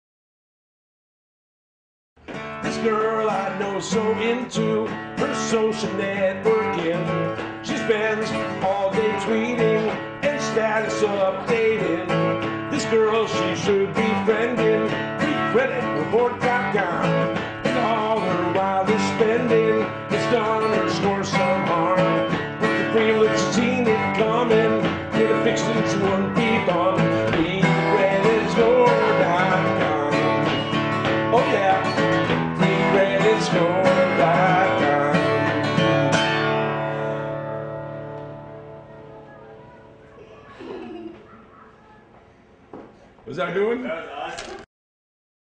YouTube- Bill Zucker and his Angels.flv
Bill Zucker and his Angels, Paula Labaredas, Misty La vina, Nicole Andrews shooting a freecreditscore.com video